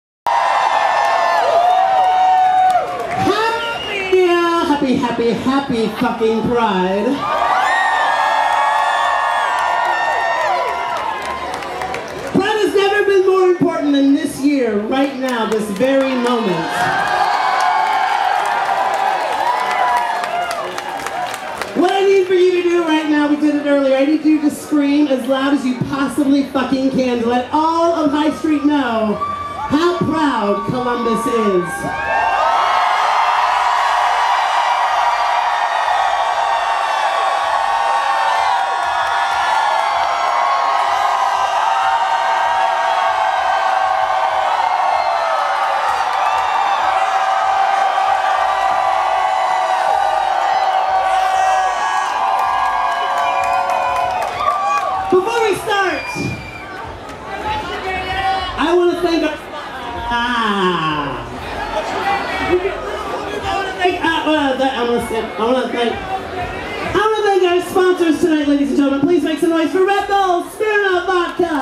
Columbus Pride - Nina West Speech 3
Nina West talks to the crowd in the Axis parking lot for Columbus Pride on June 17th, 2016!